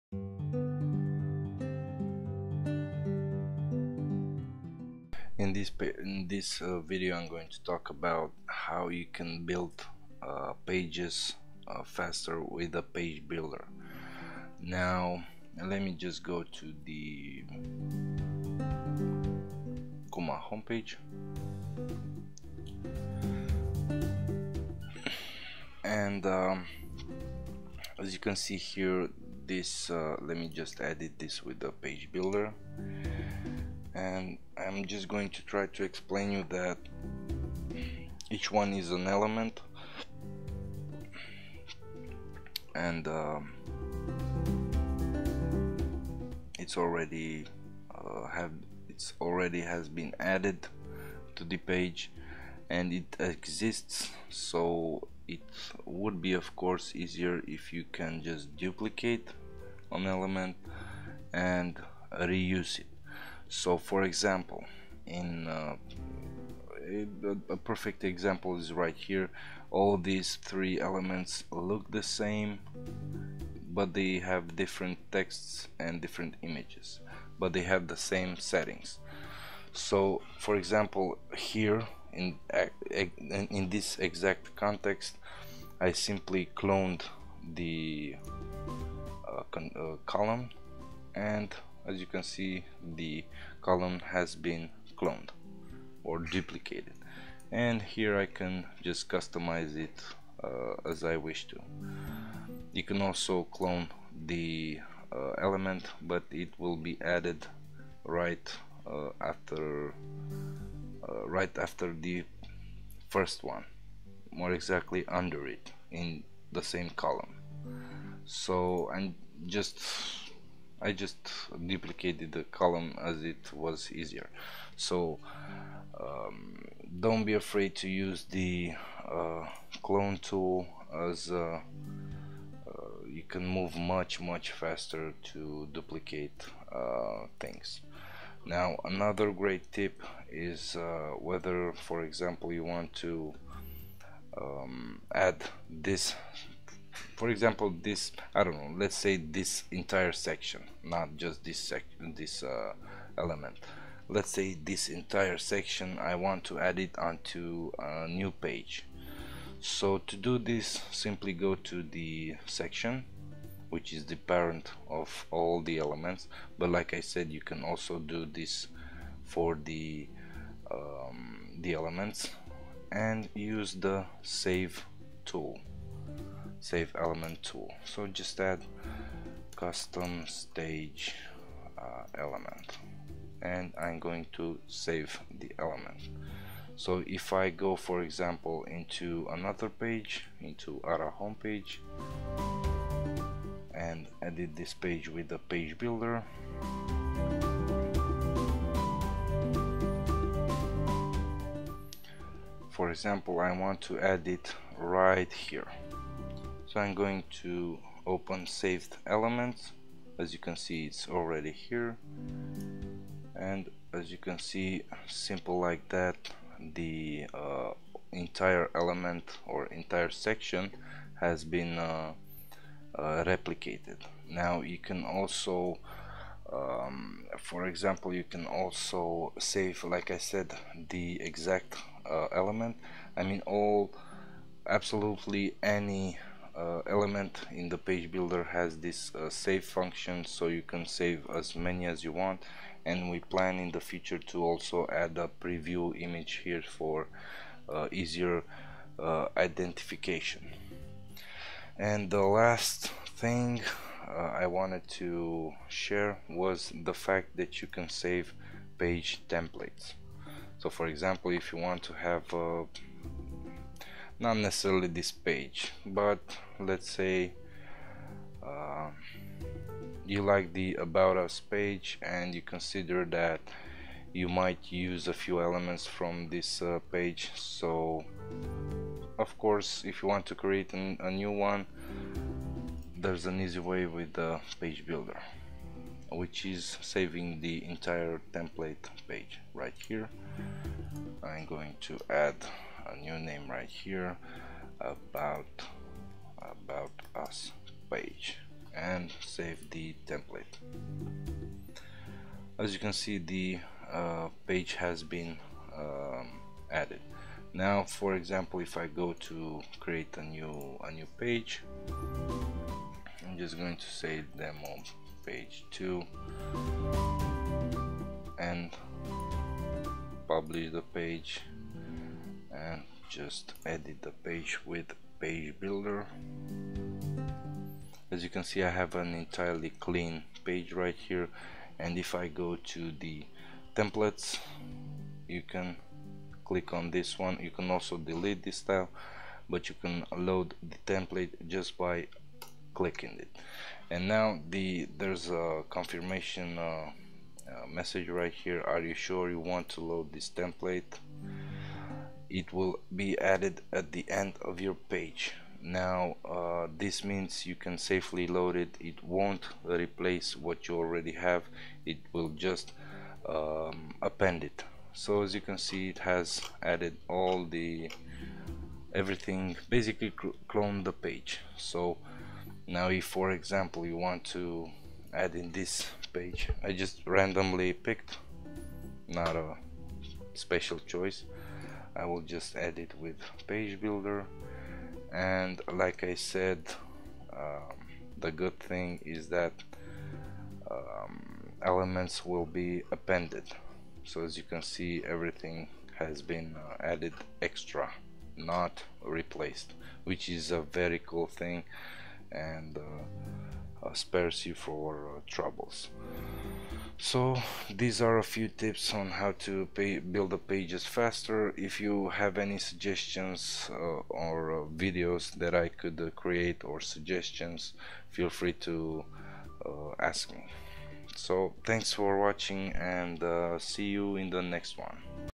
Build pages faster ( Kallyas WordPress theme v4.0 )
In this video you should find several tips and tricks on how to build pages faster with the Page builder and increase your productivity.